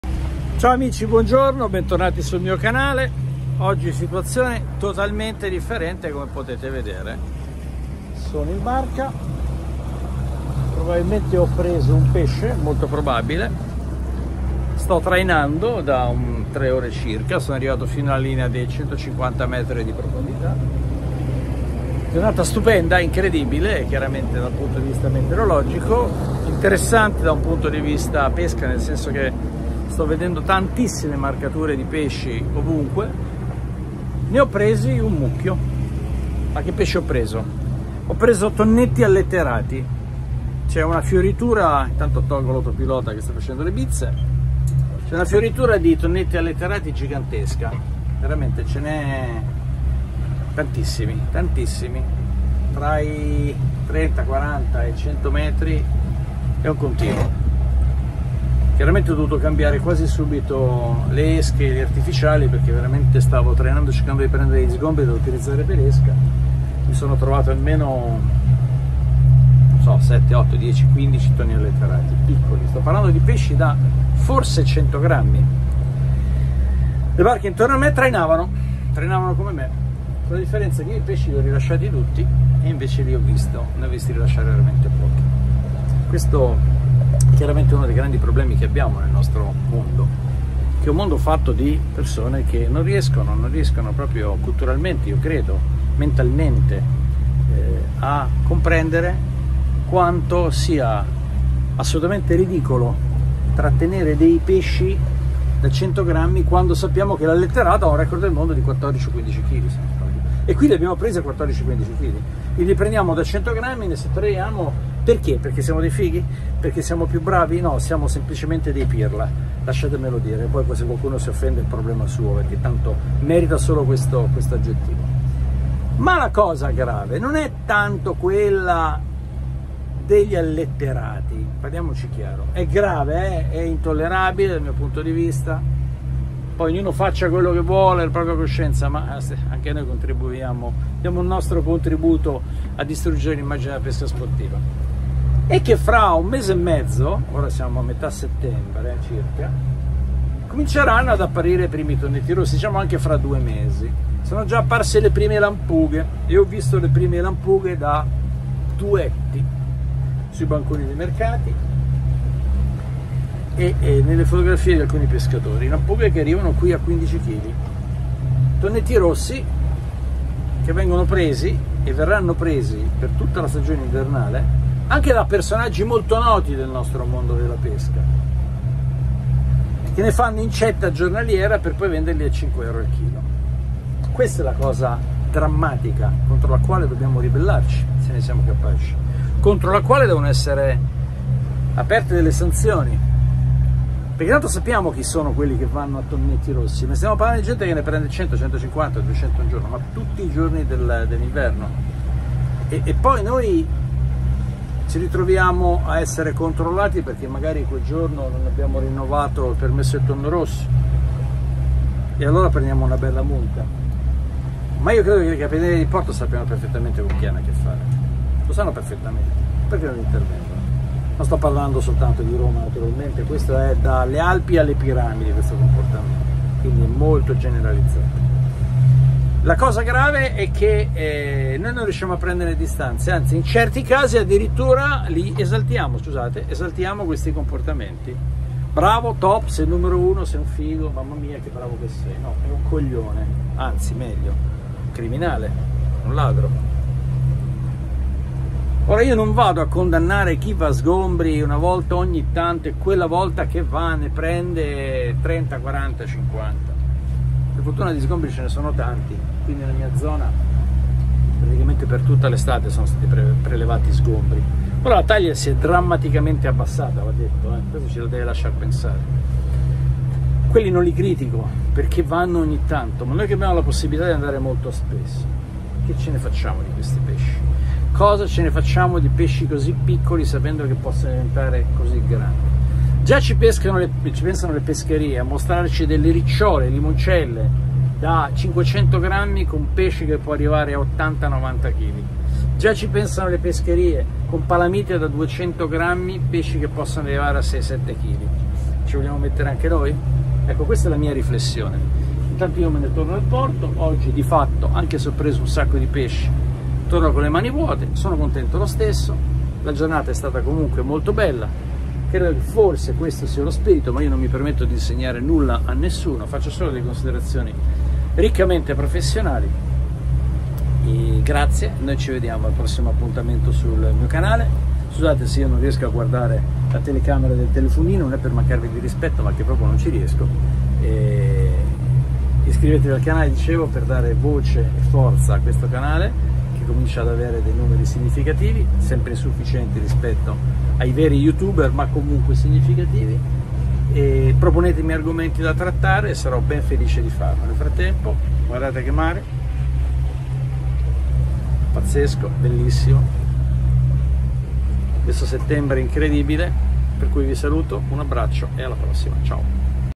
Il mare non è un supermarket!
Imparare a contenersi, conoscere le specie che catturiamo, impariamo s rilasciare e condanniamo chi fa disastri ed è impunito. Il mare non è un supermarket!